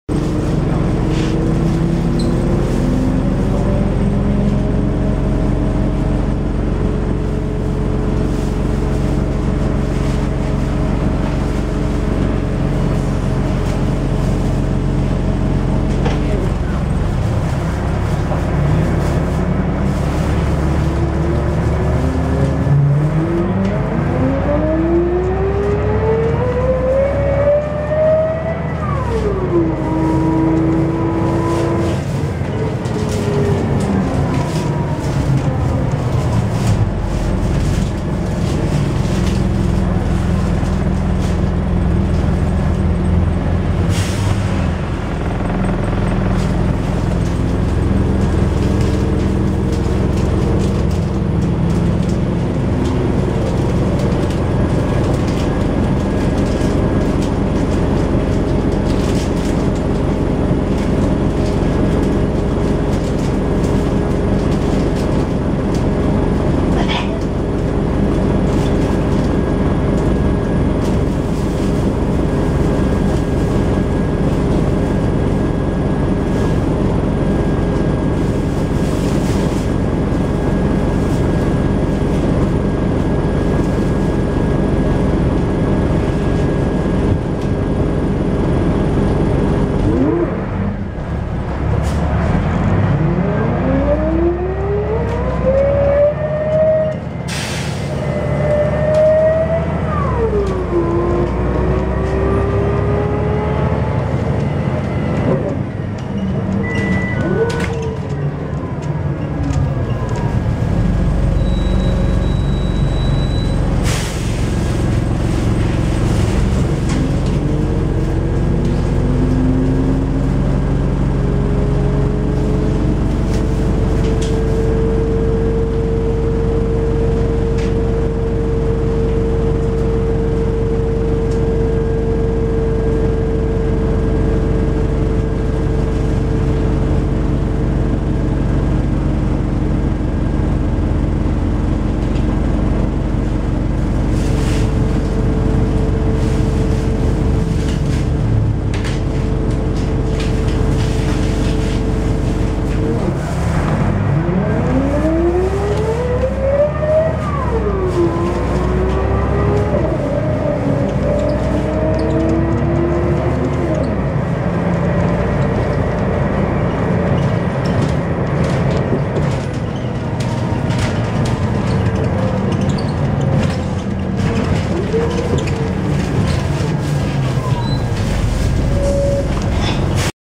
Very musical ZF - Arriva Southend DAF SB220/Plaxton Prestige 3911 (T911 KKM)
Join me aboard an extremely 'musical' DAF SB220/Plaxton Prestige of Arriva Southend on service 1 between Southend Travel Centre and Thundersley, Kenneth Road.
The gearbox notes in first gear of 3911 (T911 KKM) are exceptionally good, with even neutral having a tuneful sound to it.  A very enjoyable musical cacophony, rare on modern buses.
In part 1 we depart Southend's Bus Station, making several stops with delicious first gear notes before boarding passengers necessitated a pause in filming.
For a longer and more varied sample, try Part 3 :)